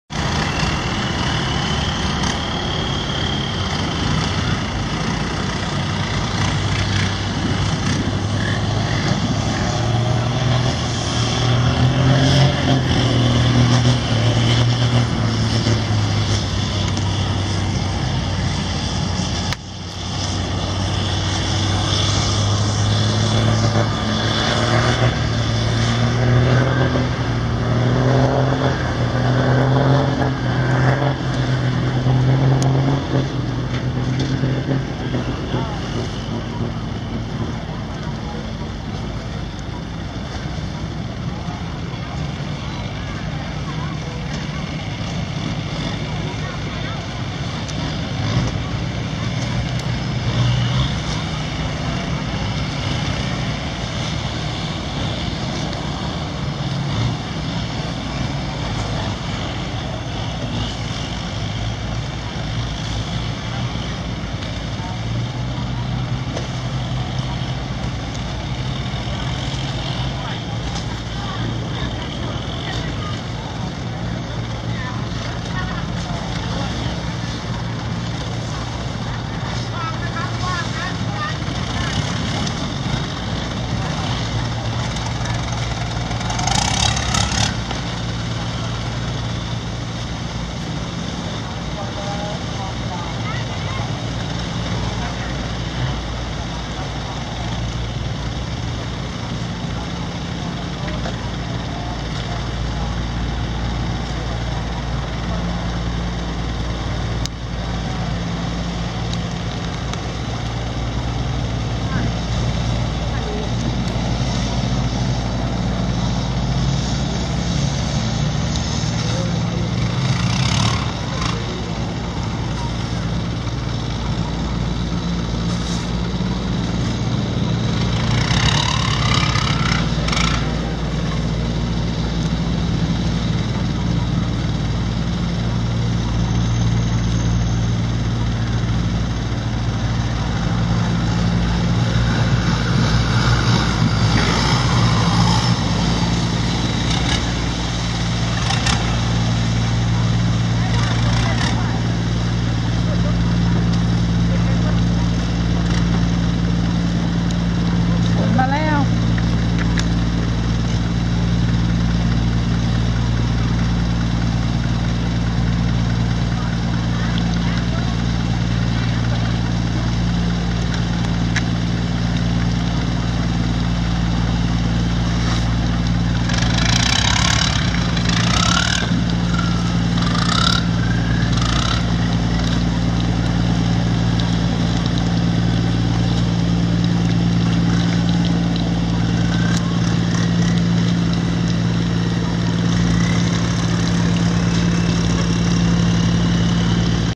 Chao Phraya River
This busy river is loud with its long- tail boats, cruise boats, and container ships. It is an exciting and a cheap way to travel around Bangkok. It starts to rain in this video as I wait at temple Arun for the next taxi boat.